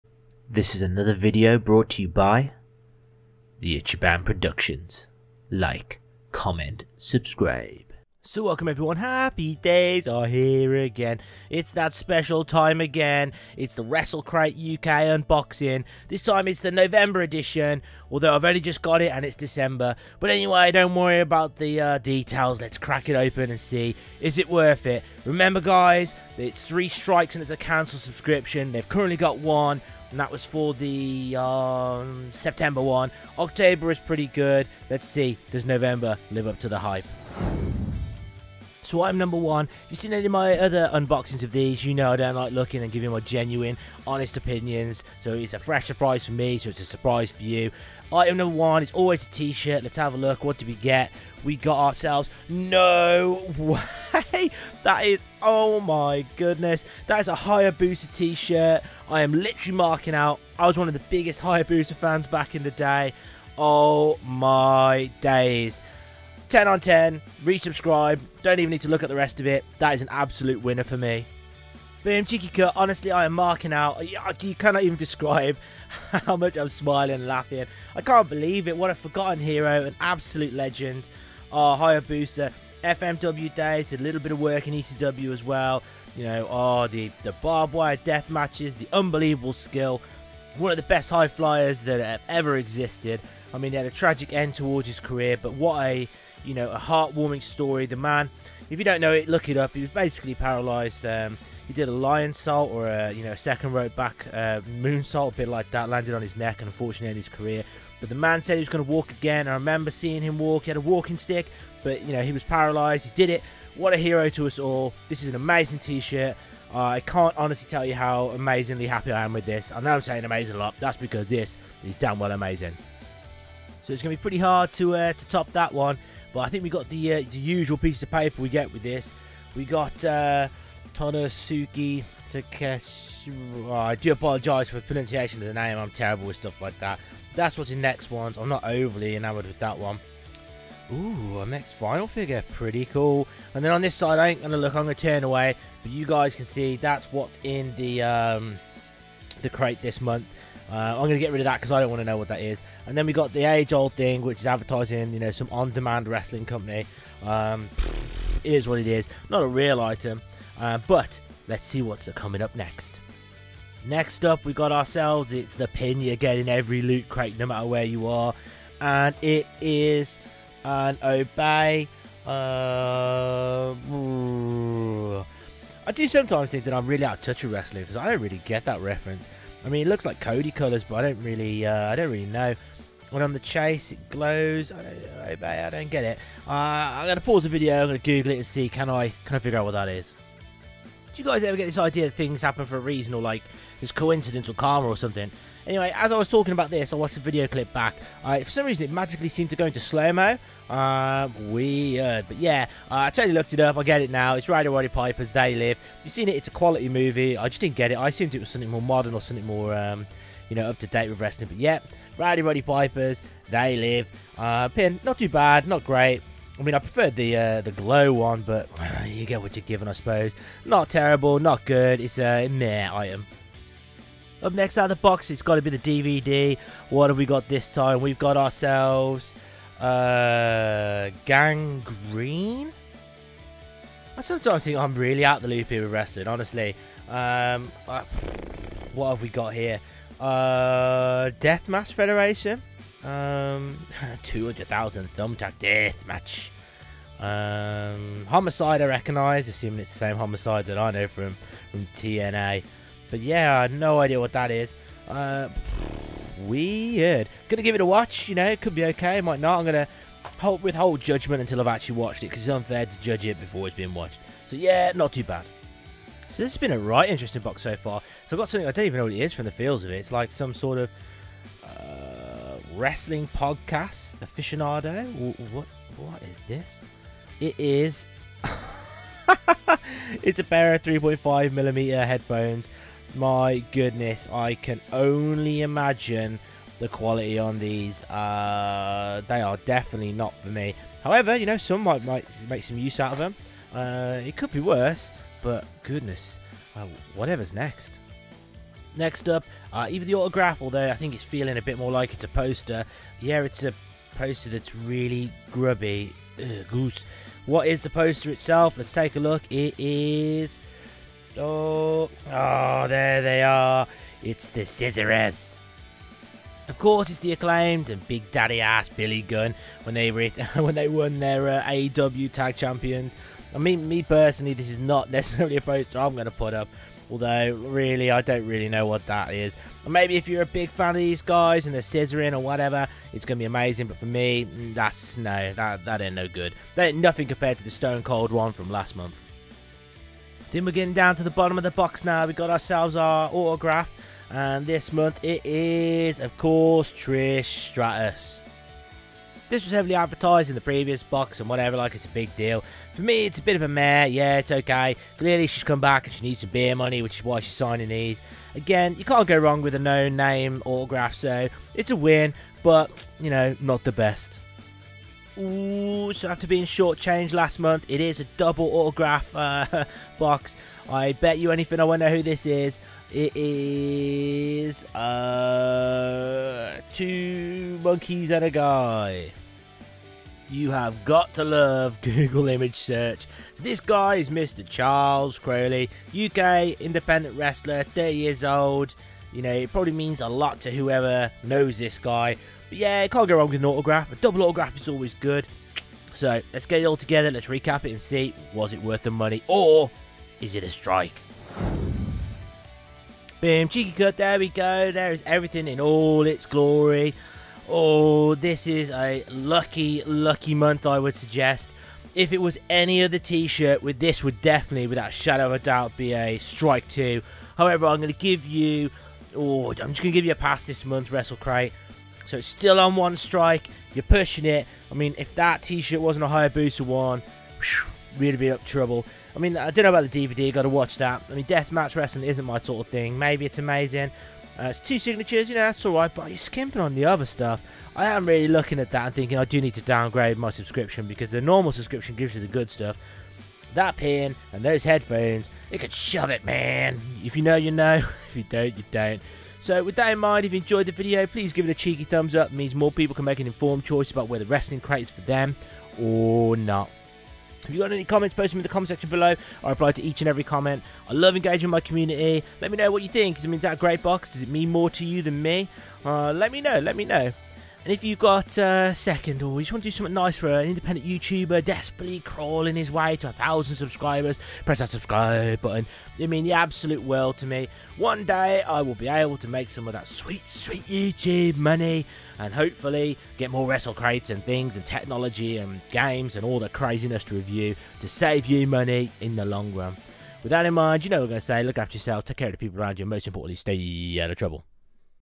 No strike this month! Wrestle Crate UK November 2022 Unboxing & Review Wrestling Loot Box
Sorry it is late. An honest unboxing and review of the November edition of the UK Wrestle Crate. An amazing T Shirt, ok autograph and a lot of other toot!